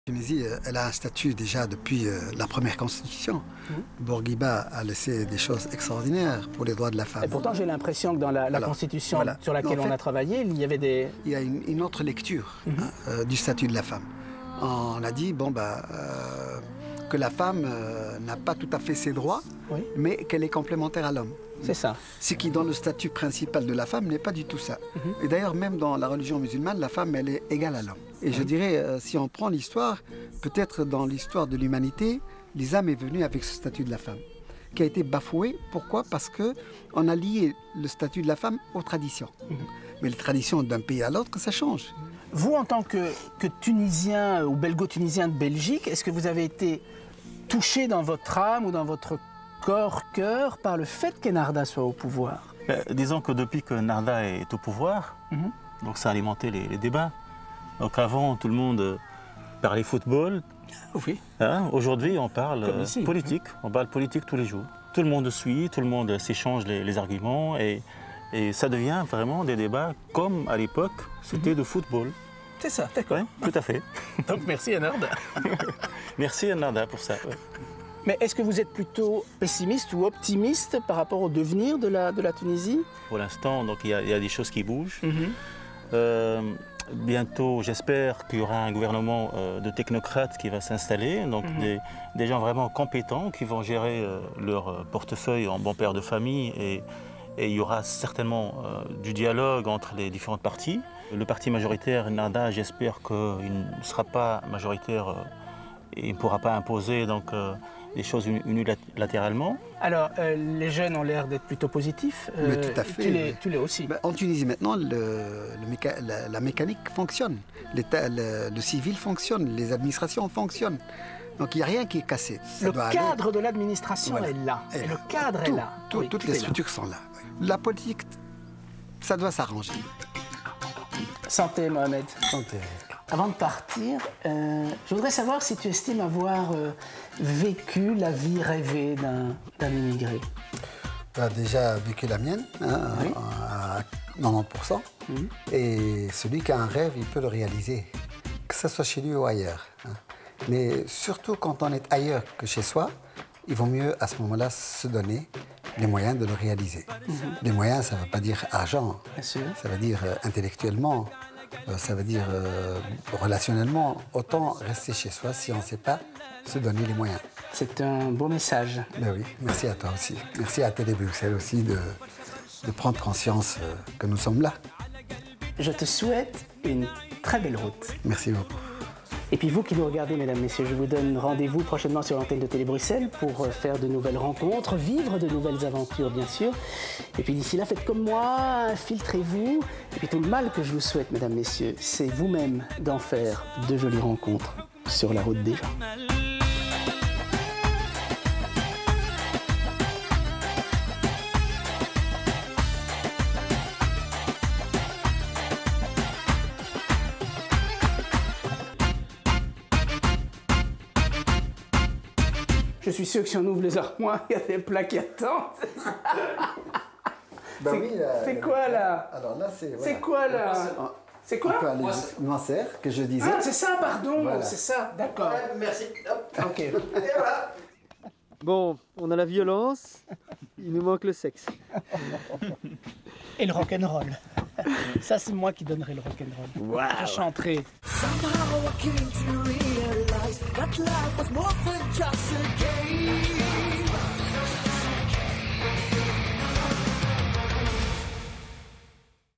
VTS 01 2
Rencontre avec Mohamed TRIKI,tunisien d’origine,il vie en Belgique depuis +de 40 ANS ?Il agent au ministère de la communauté français et artiste  peintre,il nous parle de sa vocation de sa sources d’inspiration et de la cuisine de ces origines Avec un regard sur les changements en Tunisie,avec beaucoup de bonheur.